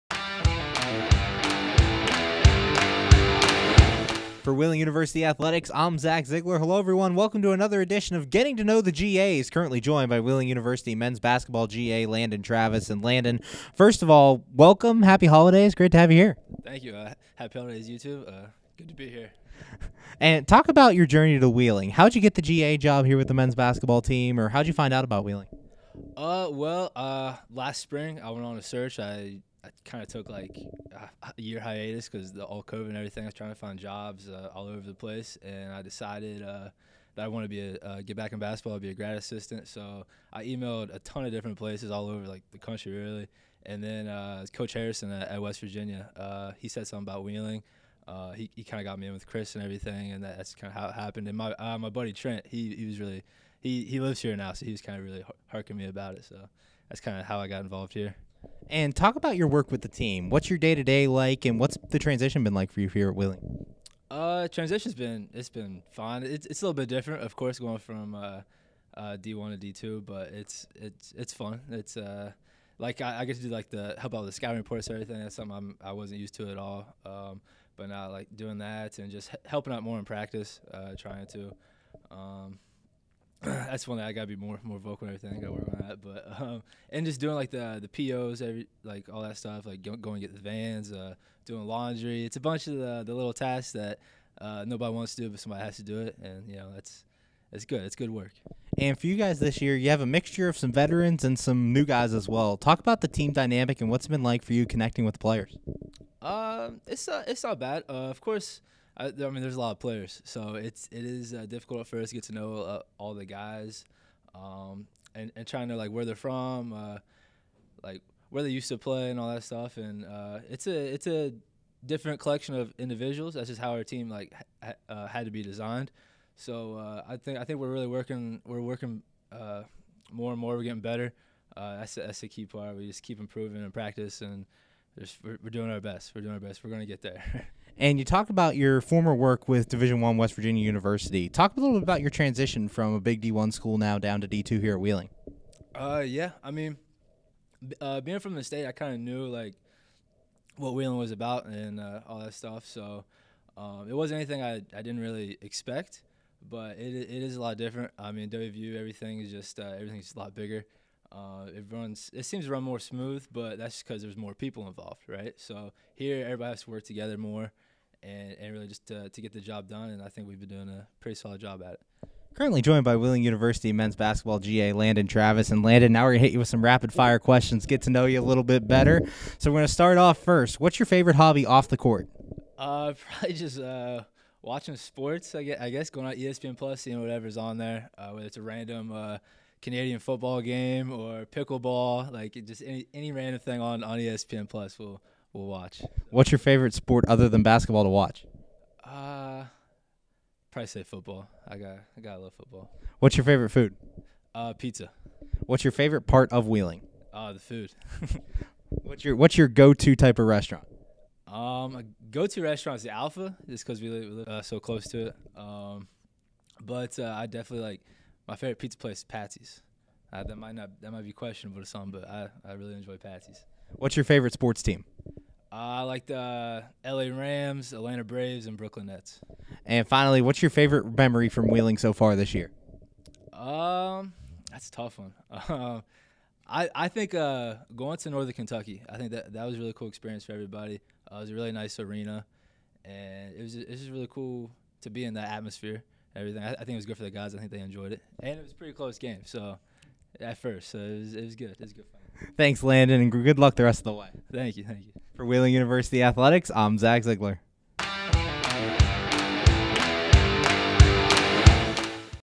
Getting to Know the GA's: Landon Travis Men's Basketball
The final Wheeling Men's Basketball GA on "Getting to Know the GA's" is Landon Travis!

He is in his first season with Wheeling, and talks about what it has been like finding his voice as a DII Assistant